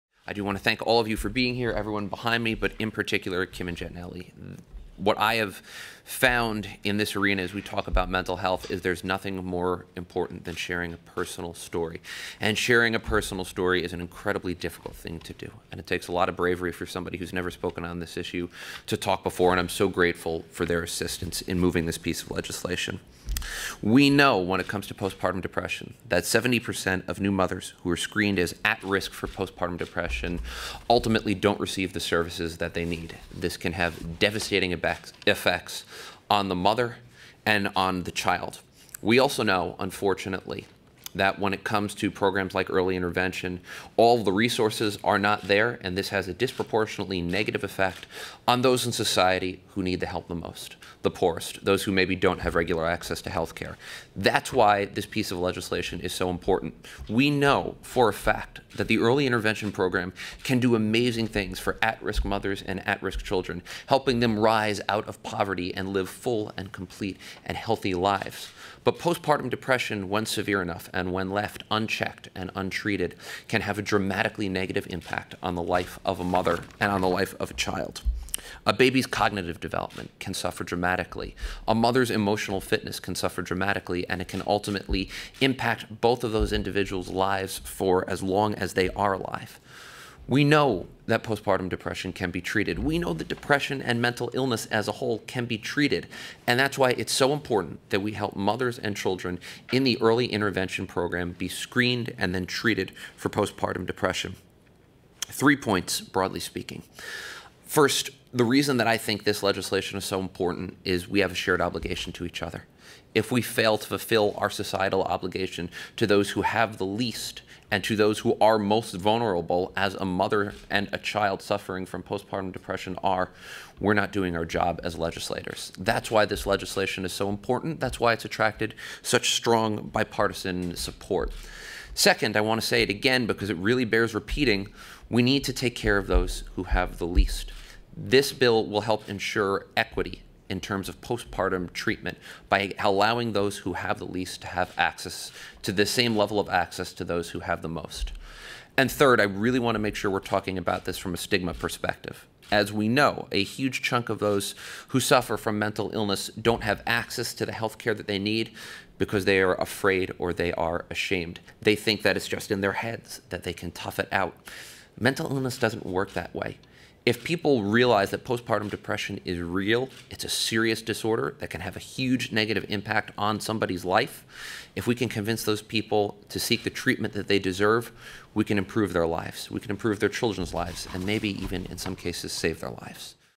Schlossberg: Postpartum Depression Treatment Works
Pa. state Rep. Mike Schlossberg joined members of the Strong Mom/Strong Baby group to ensure that infants and their mothers affected by postpartum depression have access to the services and treatment they need.